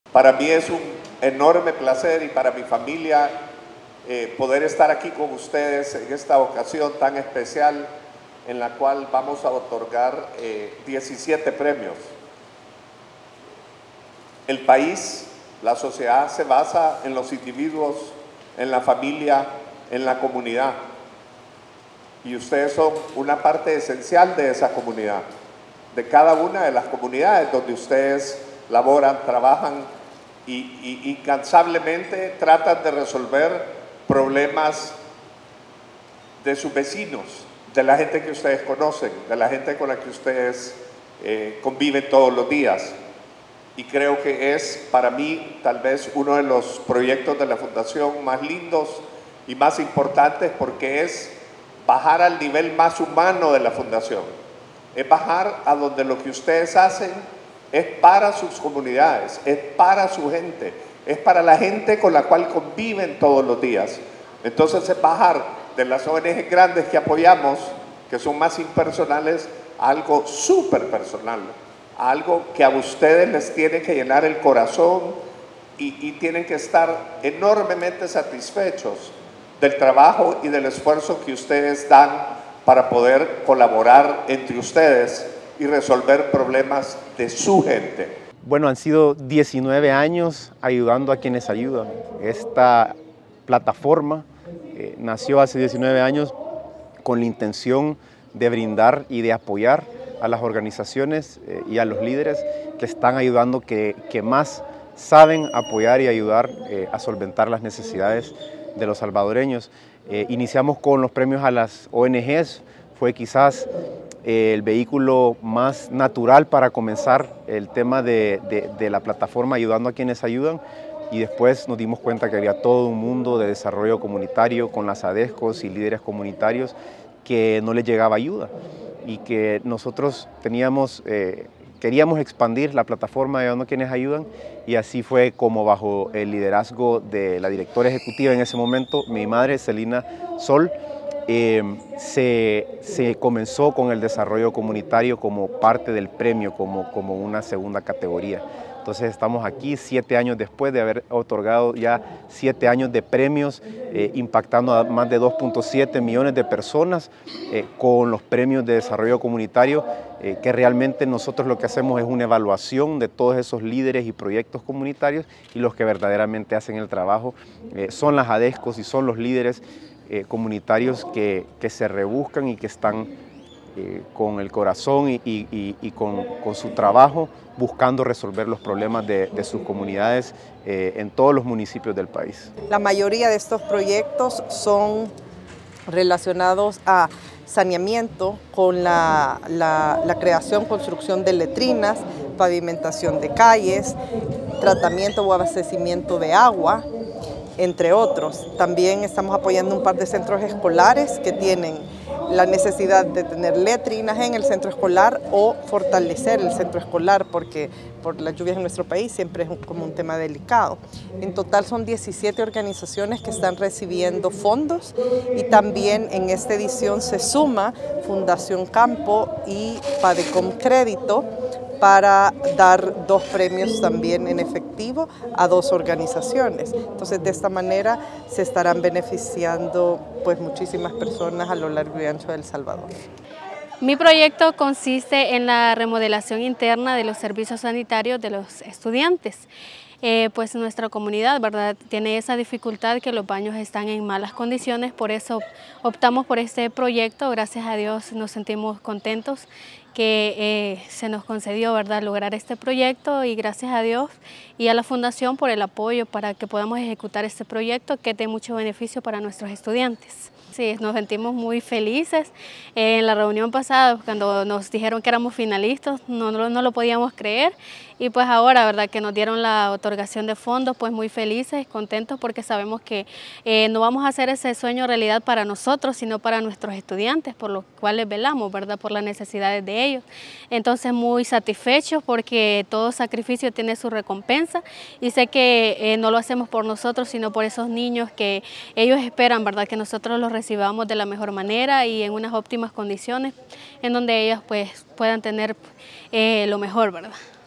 LA FUNDACIÓN GLORIA KRIETE ENTREGA, $229,200.00 EN FONDOS
LA FUNDACIÓN GLORIA KRIETE ENTREGA, $229,200.00 EN FONDOS EN LA CATEGORÍA DESARROLLO COMUNITARIO PARA PROYECTOS EN DIFERENTES DEPARTAMENTOS EN TODO EL PAÍS.